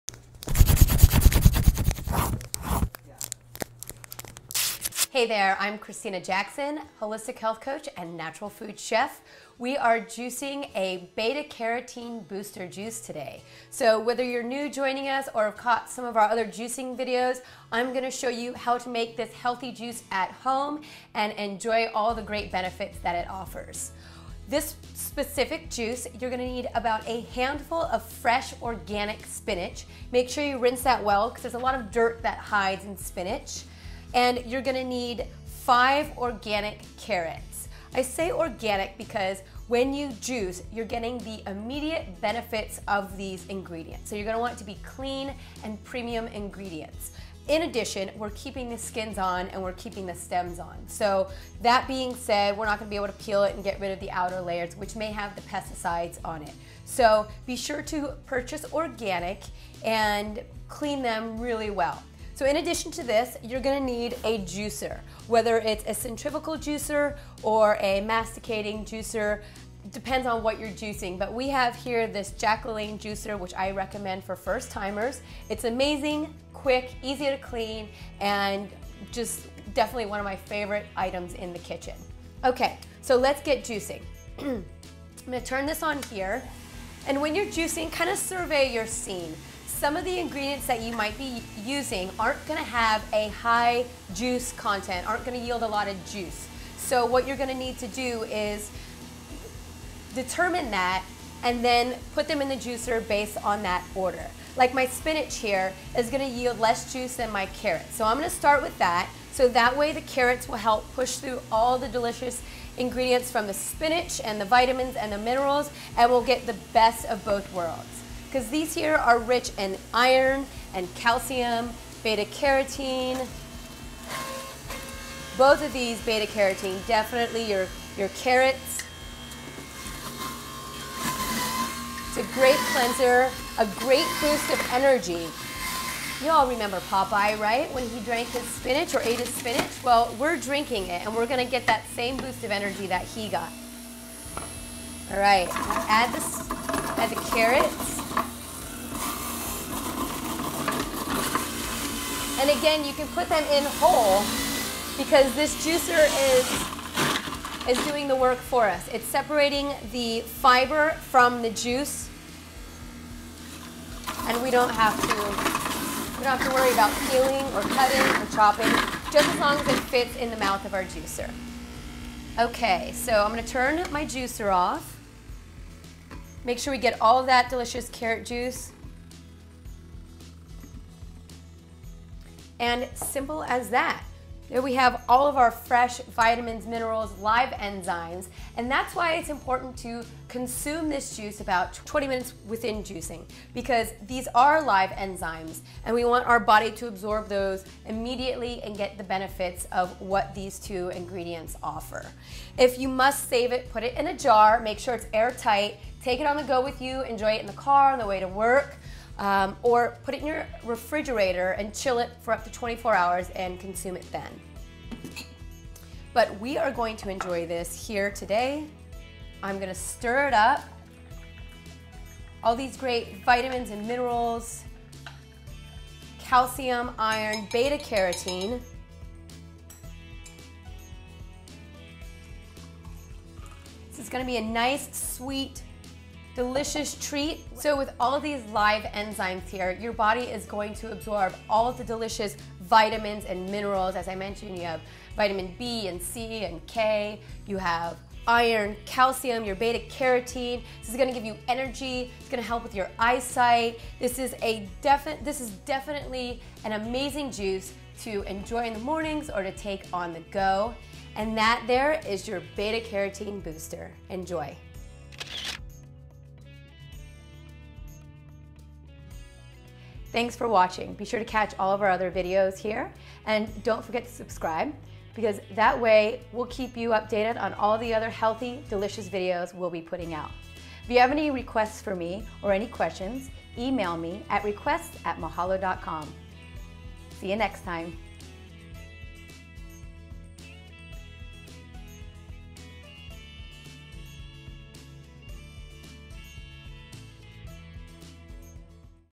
How to Make a Beta Carotene Booster Juice
Kristina Jackson, a pastry chef for Real Food Daily and a nutrition consultant with a degree from The Institute for Integrative Nutrition, shows you how to make a beta carotene booster juice.